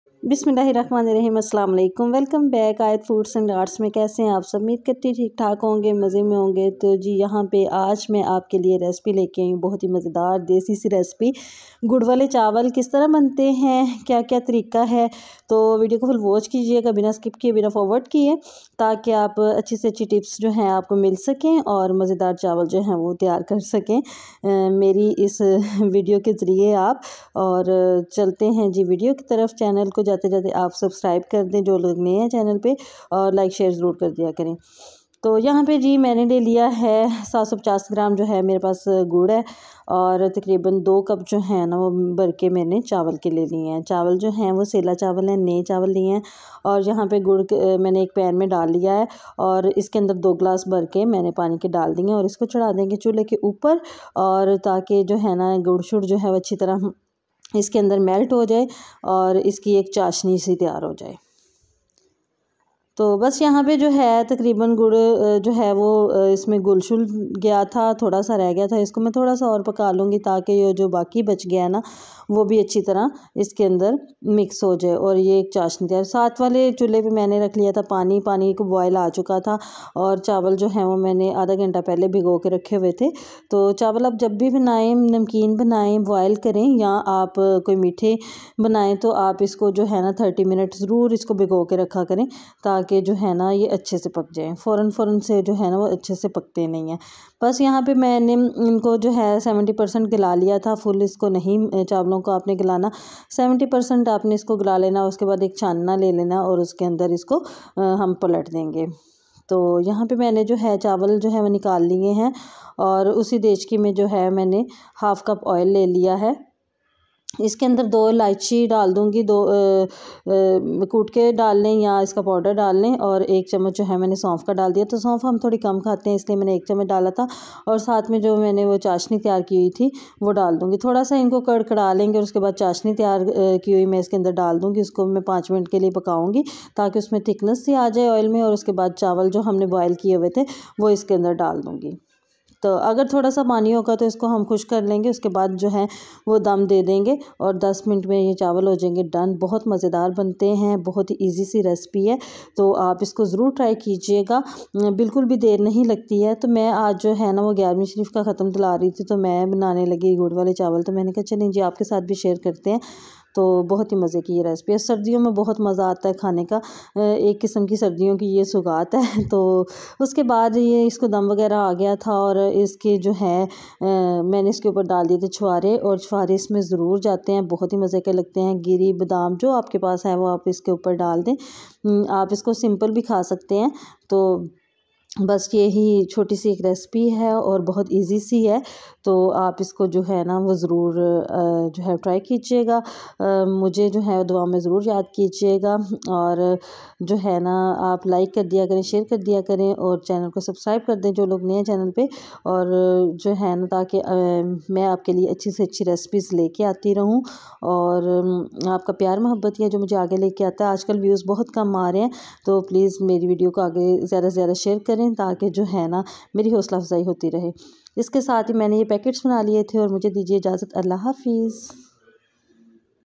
سردیوں کی سوغات گڑ والے چاولgur waly chawal Recipe by ayatfoods&art|easy recipe
Ingradants :
.Rice 2cup
Gur 750gm
.chawary As a requried
.oil 1/2cup
Alachi 2
.sonf 1tbsp